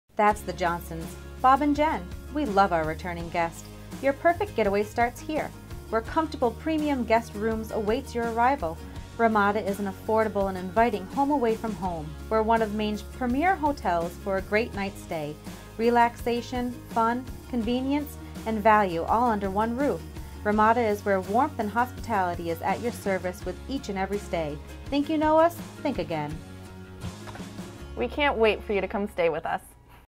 Hotel Sample 1
Commercial I completed for my company. PremiereCC. Our Deadline for this type of product is ~3 hours. This is an example of what can be done in that time frame.